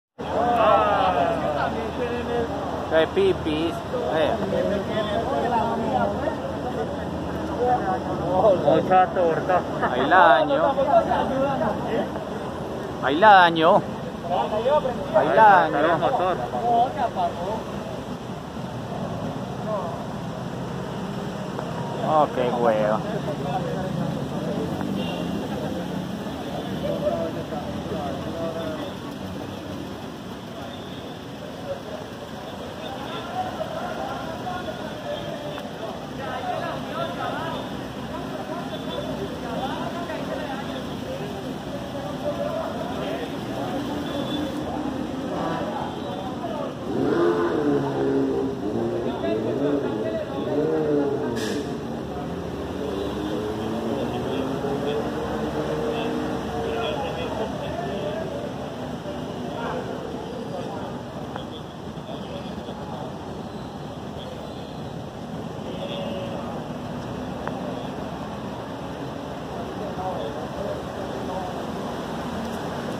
feria de Ganado en Medellín
Deprimido Feria de ganado. lo raro sería que no se inunde cuando llueva.